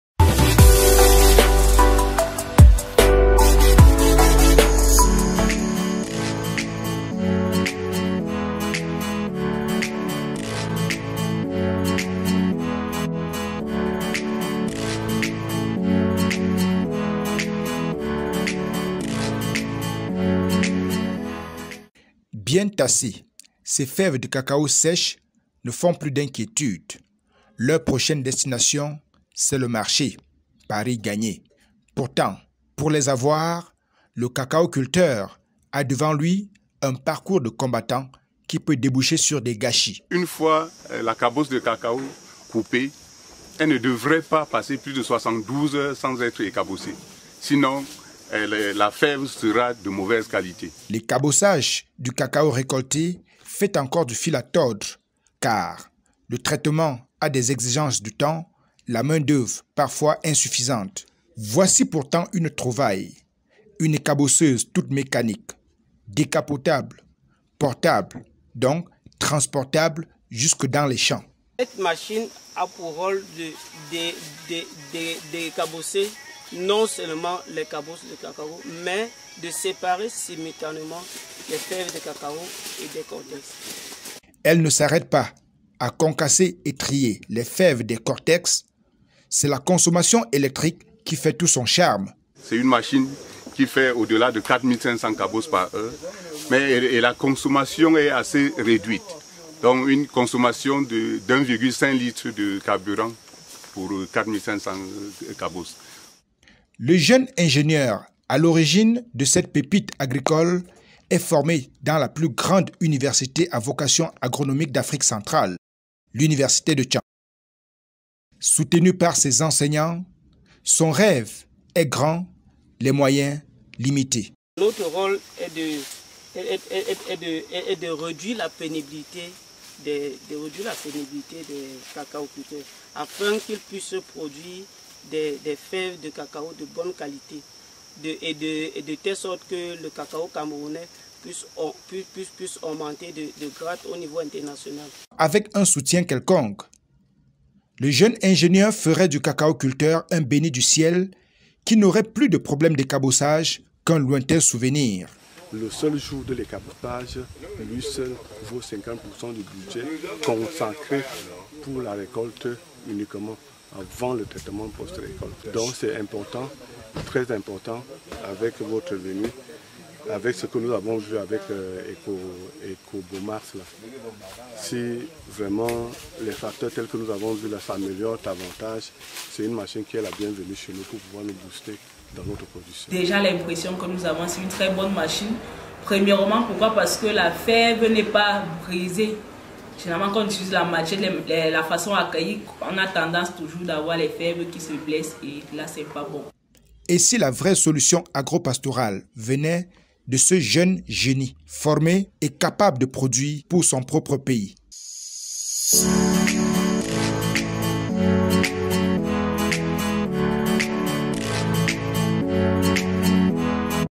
Ecabo Max: Et le cacao a trouvé sa machine à écabosser.
Michel Mongoué est un jeune ingénieur formé à la Fasa de Dschang. il propose aux cacaoculteurs une invention qui revolutionner  l'ecassobossage du cacao.Desormais  le traitement post-recolte du cacao peut se faire avec moi si de main de œuvre ,un gain e temps et une assurance qualité pour les ferves.les produits de l'Université  de Dschang apportent ainsi leur rôle dans le développement du monde agro-pastoral.